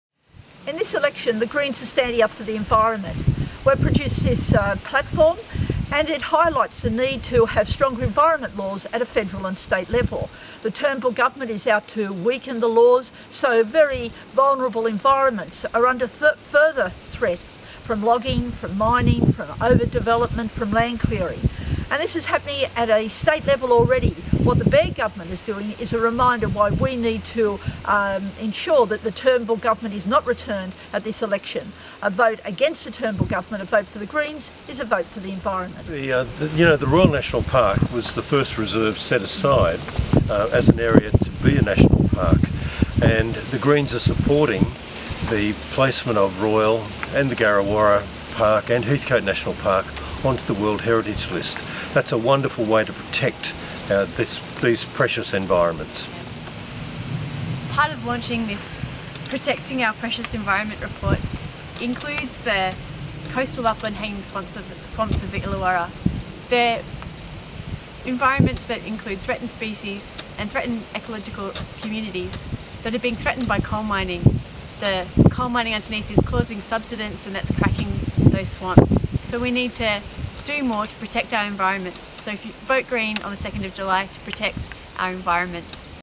Protecting Our Precious Environment
Cath Blakey, Phil Smith and senator Lee Rhiannon launch the Greens environmental policy for the 2016 in the Royal National Park.